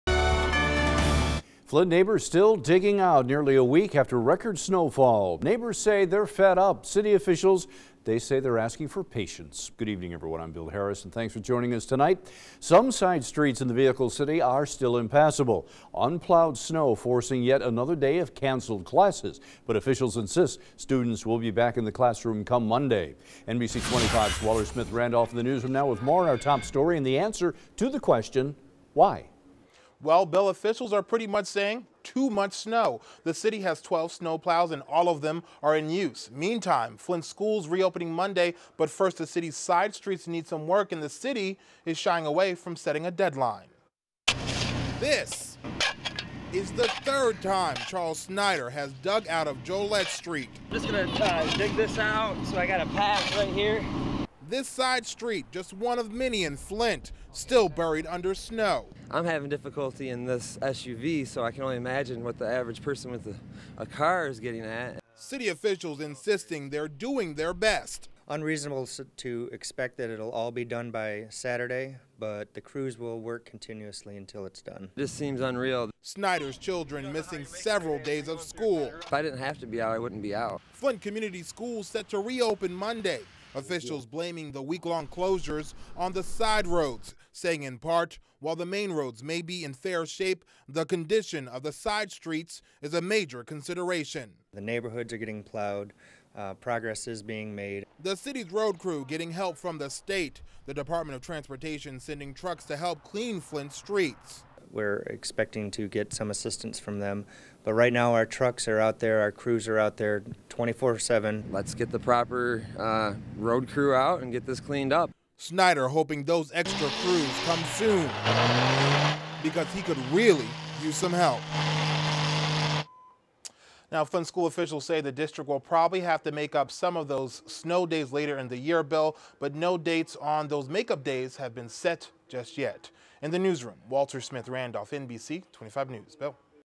Flint neighbors continue to dig out; city schools closed for fifth day
It's been five days since record snowfall hit Flint. Neighbors are still digging out and Flint students have yet another day off. NBC25's Walter Smith-Randolph reports.